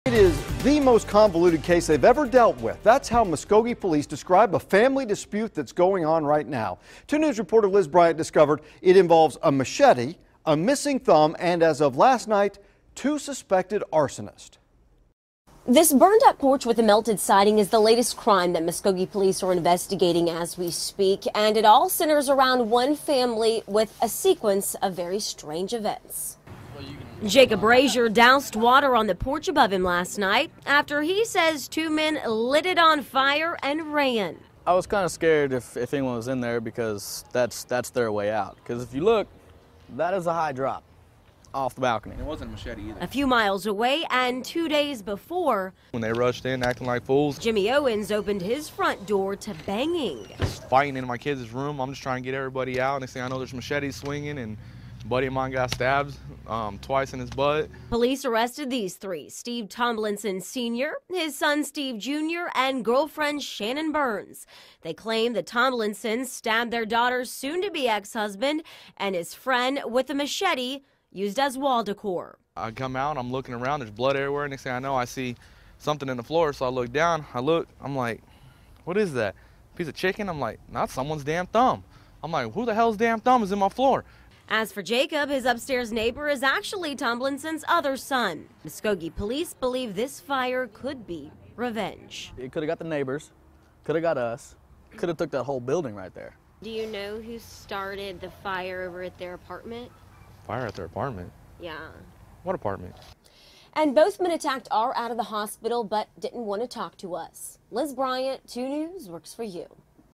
Machete attack during Muskogee family dispute
2News Works for You at 10p